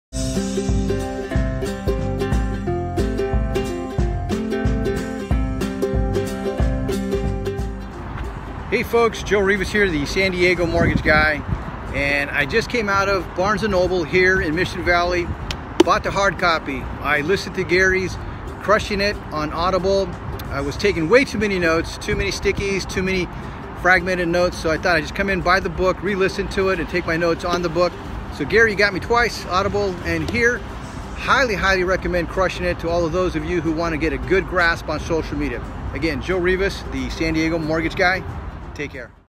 Buying Crushing It by Gary Vee
San Diego Mortgage Guy Shops Local at Barnes & Nobel and Buys Crushing It by Gary Vee!